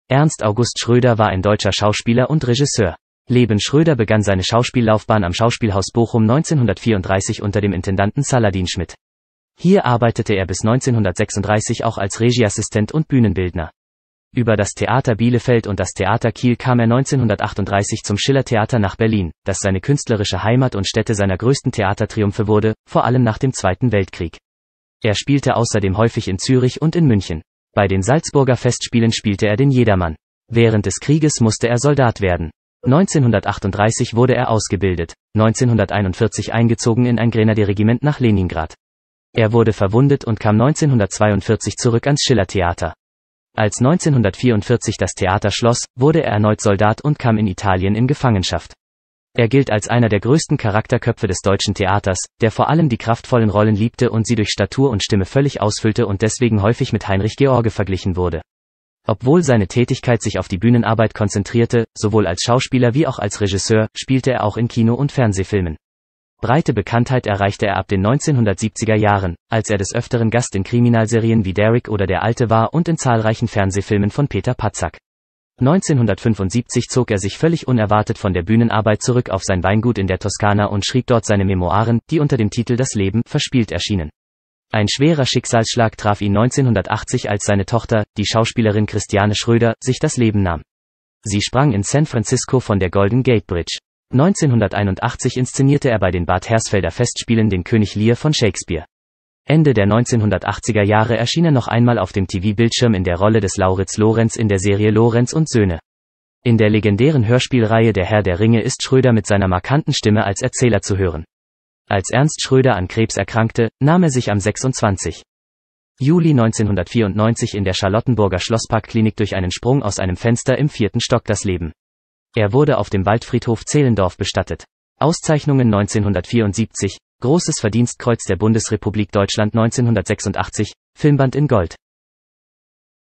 Ernst Schröder Schauspieler Biografie - Deutsche Schauspieler
Ernst August Schröder  war ein deutscher Schauspieler und Regisseur.
Leben
Schröder begann seine Schauspiellaufbahn am Schauspielhaus Bochum 1934 unter dem Intendanten Saladin Schmitt. Hier arbeitete er bis 1936 auch als Regieassistent und Bühnenbildner. Über das Theater Bielefeld und das Theater Kiel kam er 1938 zum Schillertheater nach Berlin, das seine künstlerische Heimat und Stätte seiner größten Theatertriumphe wurde, vor allem nach dem Zweiten Weltkrieg. Er spielte außerdem häufig in Zürich und in München. Bei den Salzburger Festspielen spielte er den Jedermann.